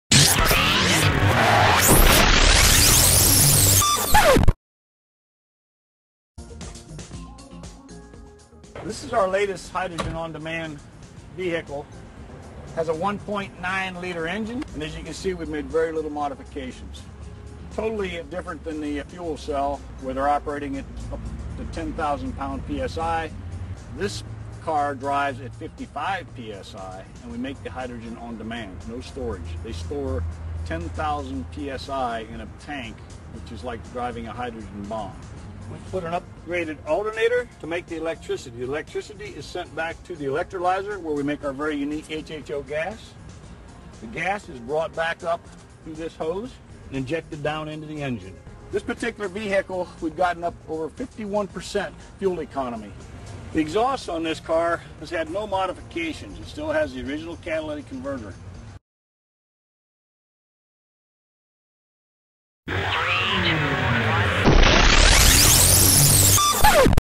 HHO Aquygen hybrid vehicle
Hey all this what the video that got me initially interested in HHO assisted systems.

AH